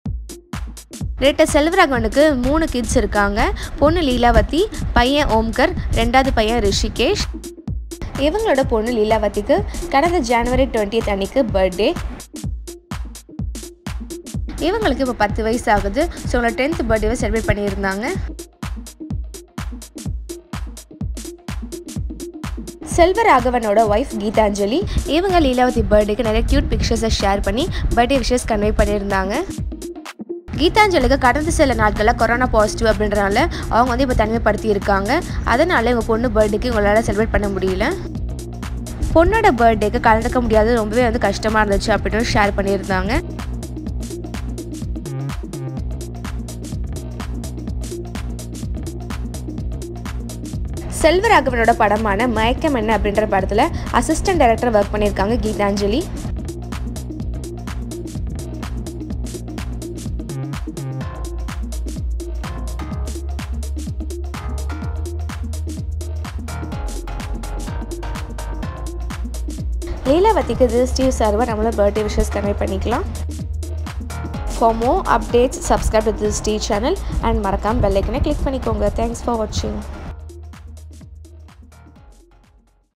Selvaraghavan Daughter 10th Birthday Celebration 🥳 | Dhanush | Aishwarya | Gitanjali
Director Selvaraghavan Daughter Lillavathy Celebrates her 10th Birthday on Jan 20 2022 . Family Cute Pictures ..